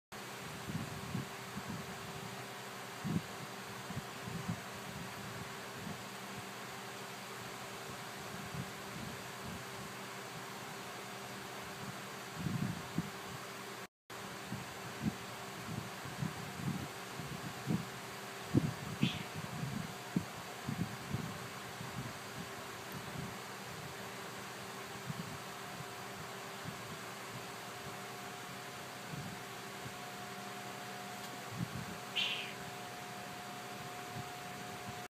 Dave Playing the Invisible Drums
Dave the cat plays his invisible drum